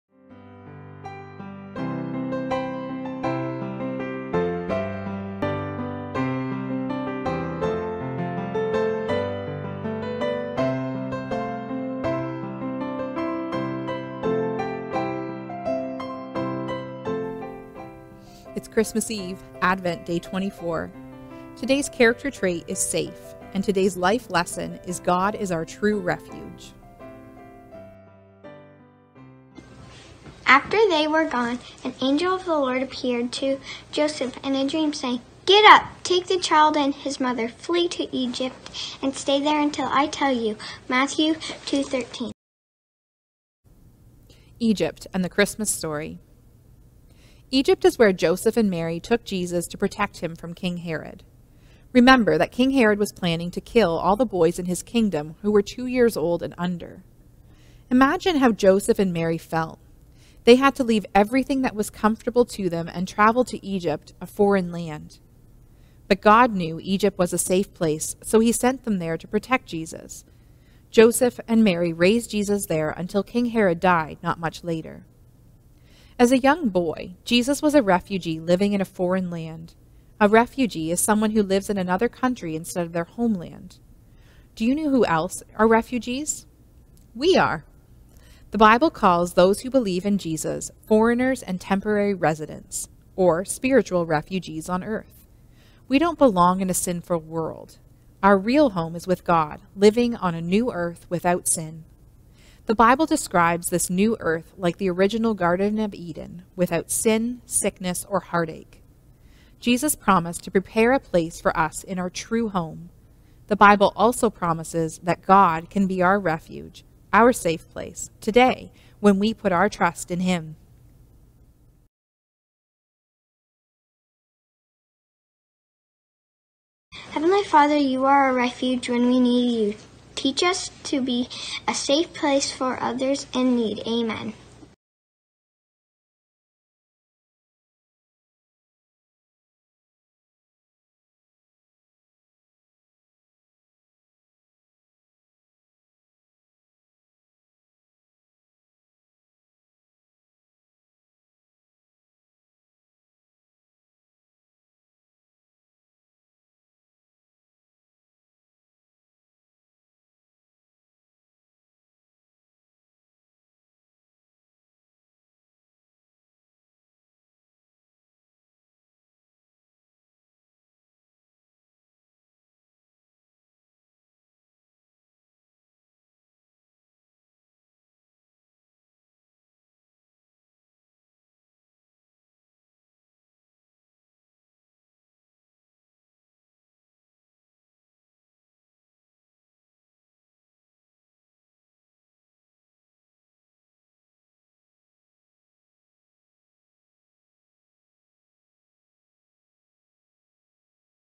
Advent Day 24
Egypt and the Christmas Story
Character Trait: Safe
Life Lesson: God is our true refuge